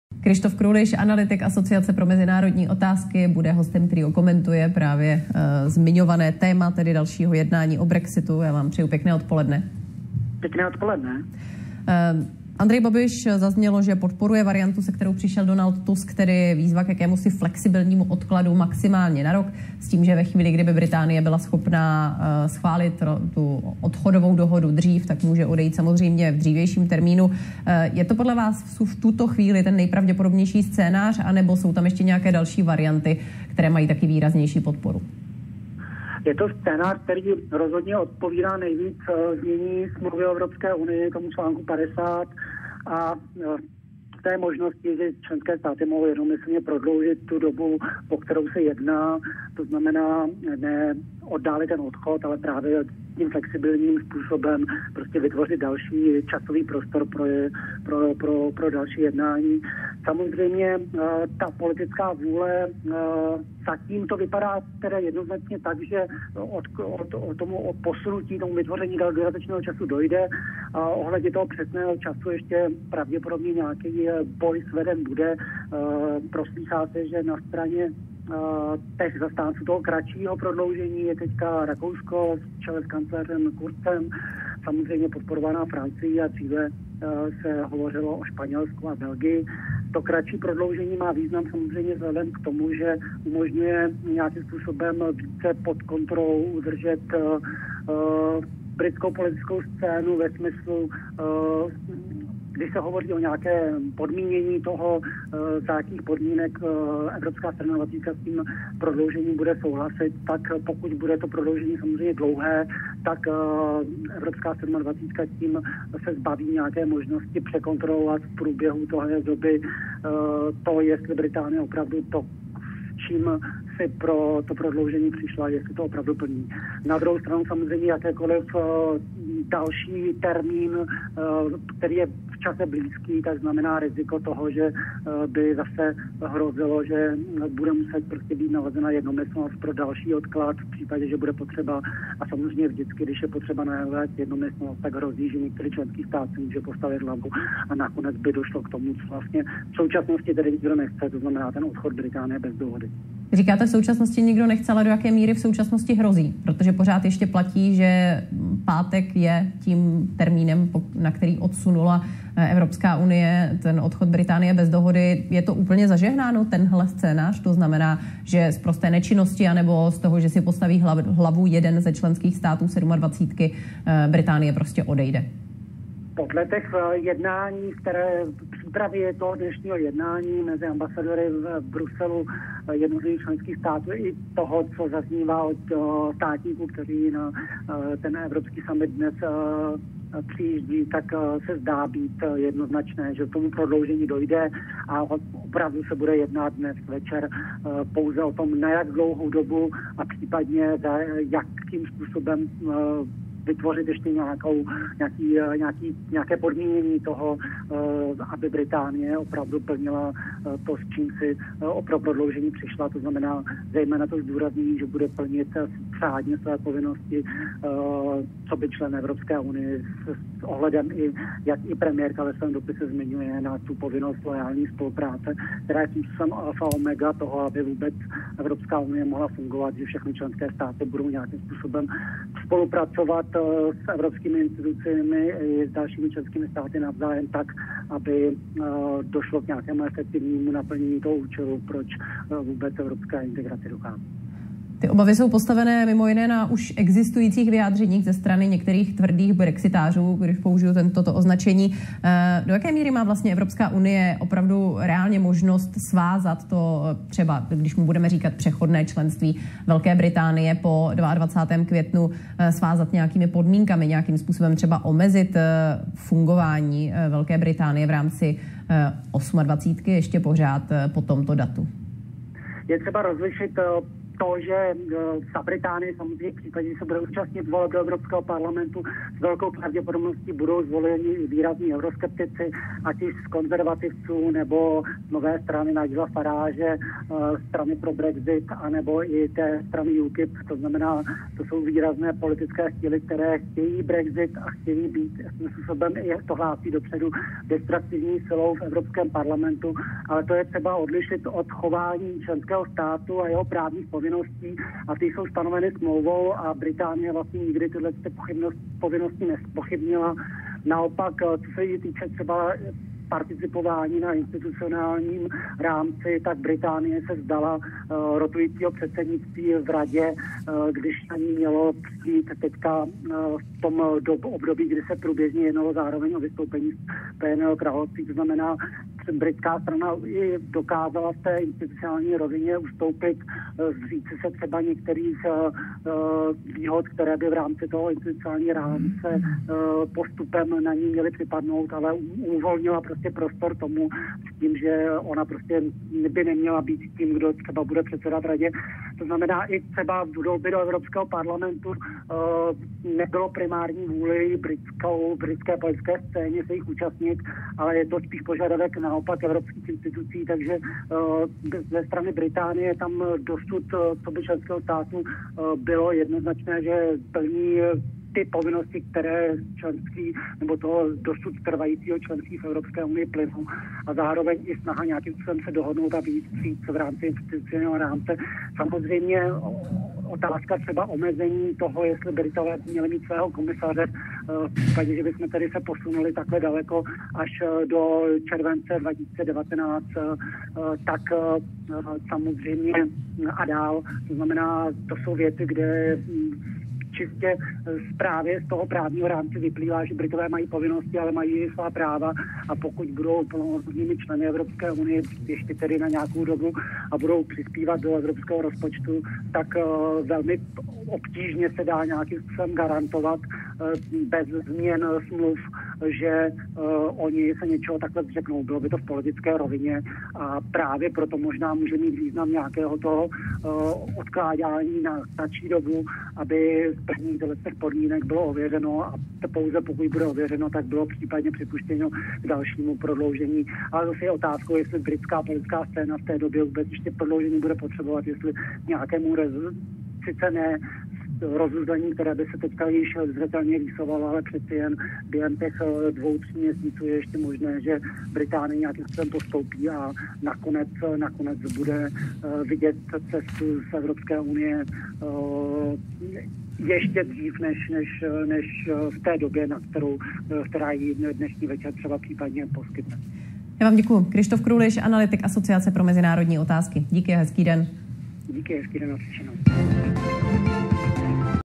V Bruselu začíná summit EU o brexitu. Co od něj čekat?
V Bruselu začíná summit Evropské Unie. Lídři projednají žádost o odklad brexitu. Ten by podle britské premiérky Theresy Mayové poskytl čas na ratifikaci dohody o vystoupení  a zajistil hladký odchod. Co od summitu čekat a jaké jsou možné scénáře vývoje? O tom promluvil VE Studiu ČT24 Kryštof Kruliš.